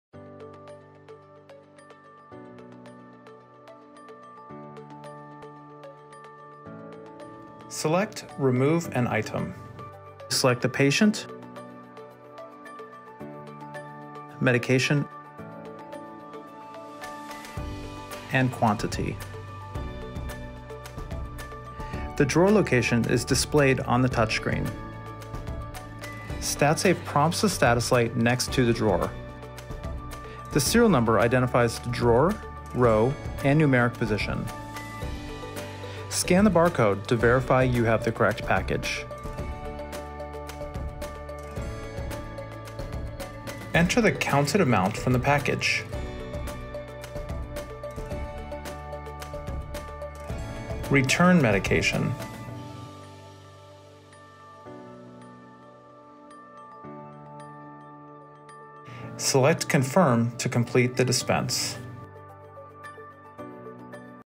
Dispense Meds - Open Matrix
Learn how to dispense a medication from Open Matrix drawer.